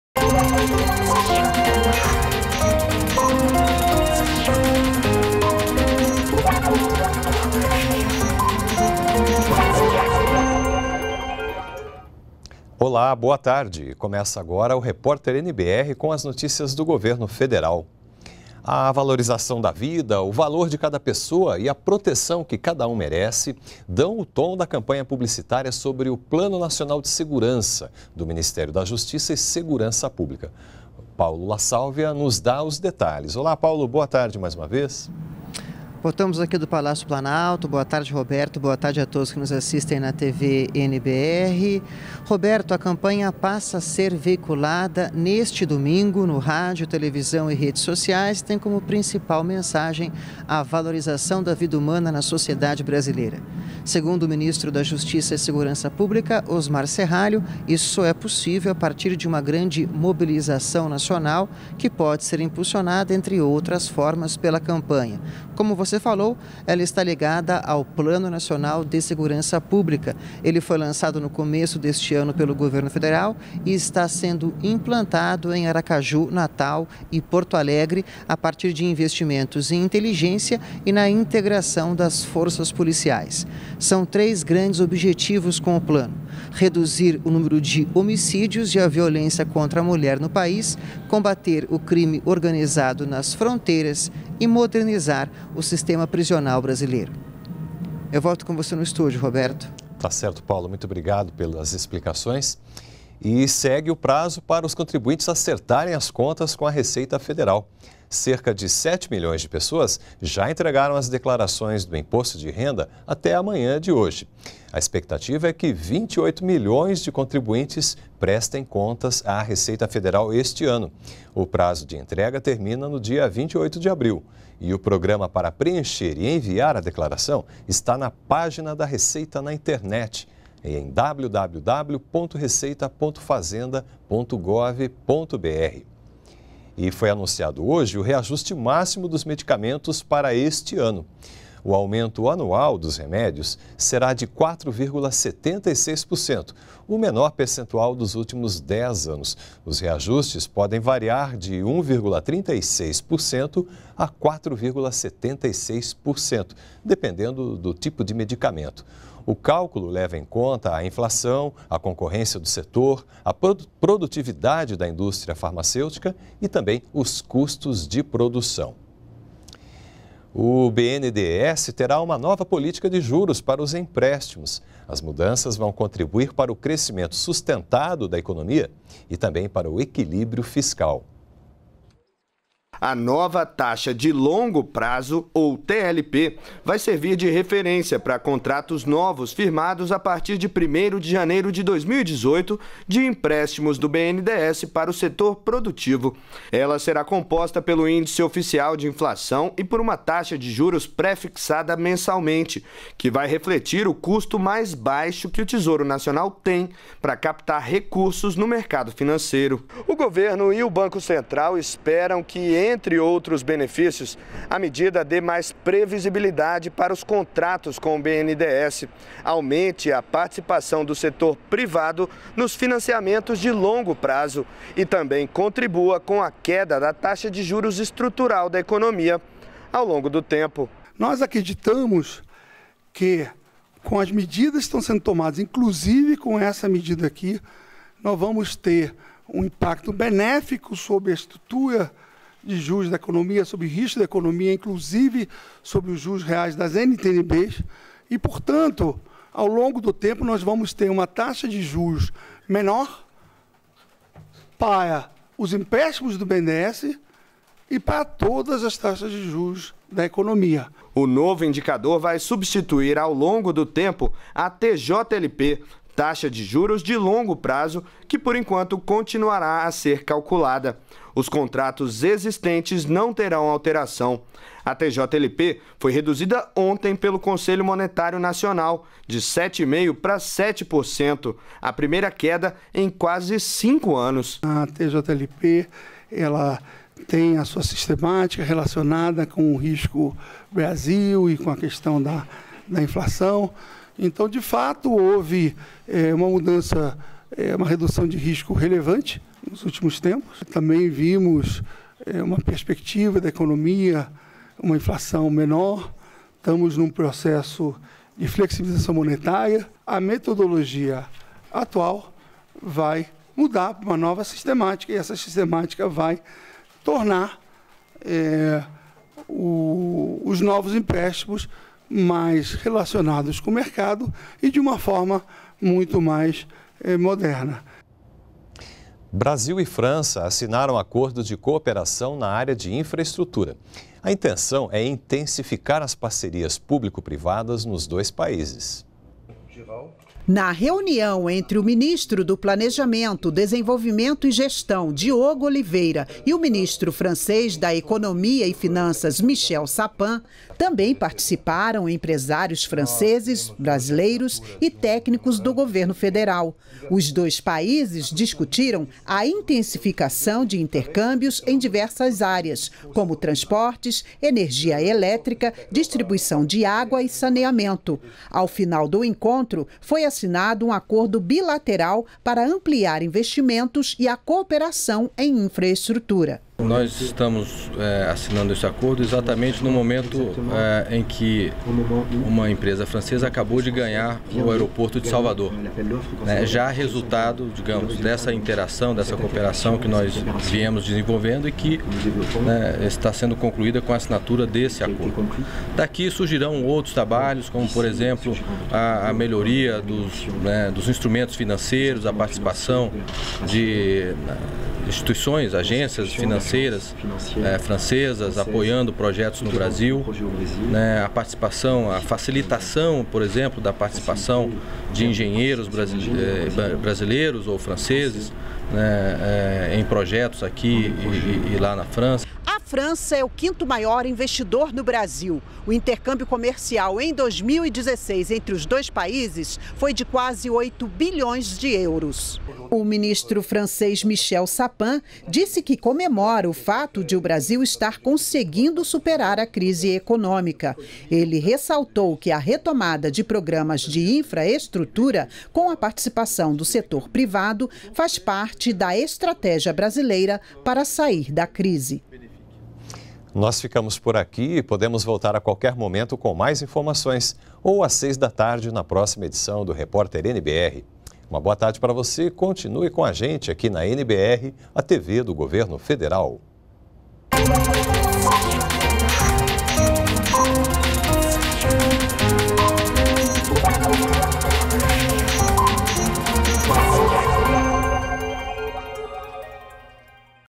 Ministério da Justiça lança campanha sobre o Plano Nacional de Segurança
REPÓRTER NBR 17H - 31.03.17: O Ministério da Justiça lançou campanha publicitária sobre o Plano Nacional de Segurança. Ação tem como mote a valorização da vida. Confira também: cerca de sete milhões de pessoas já entregaram a declaração de imposto de renda. Prazo se encerra dia 28 de abril. E mais: Ministério da Saúde divulga percentual máximo para o reajuste do preço de medicamentos - cerca de 4,76%.